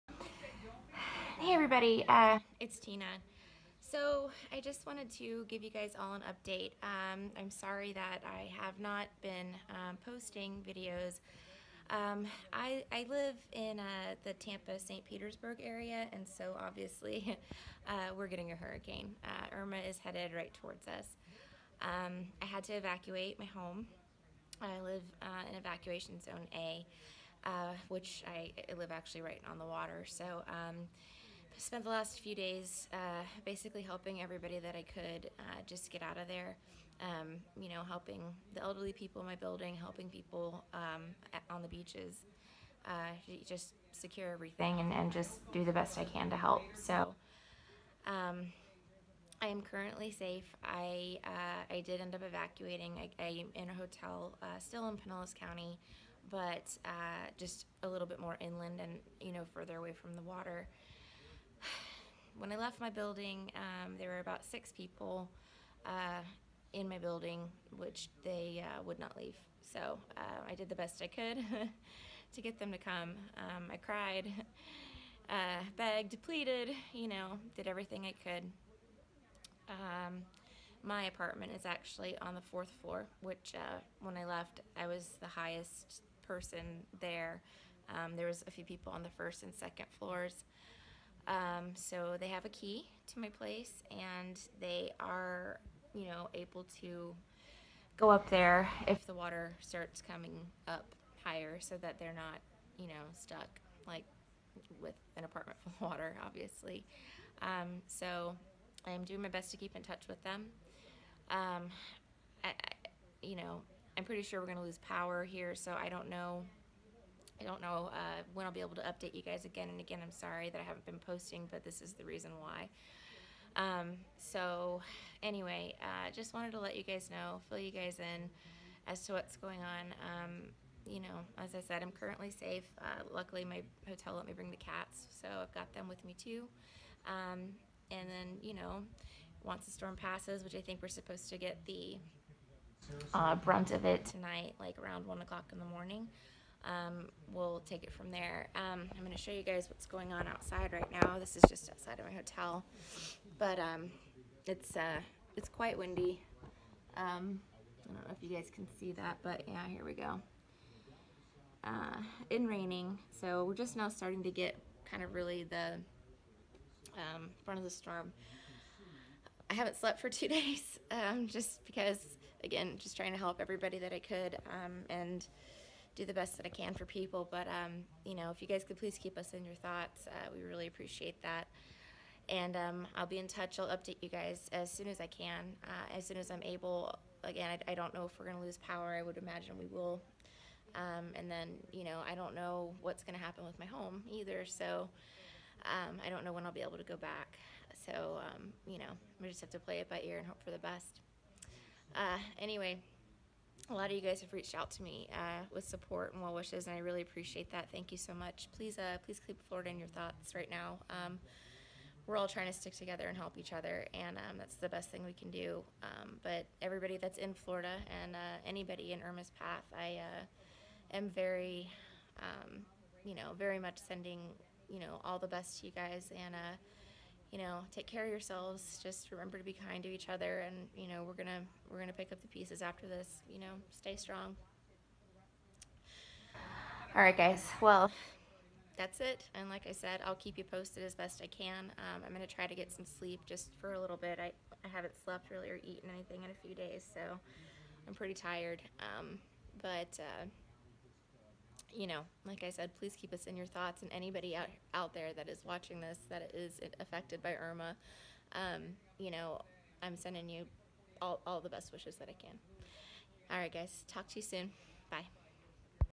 Hurricane Irma update from Florida
Hi everyone. Just wanted to give you all an update. I live in the Tampa Bay Area and was evacuated from my home. I am safe but please keep everyone in Irmas path in your thoughts.

If you are waiting for a reading Or response to an email I will get it to you as soon as I can.

To those who have reached out, thank you. I'll update you all as soon as possible.

Lots of love and light to all of you.

Tina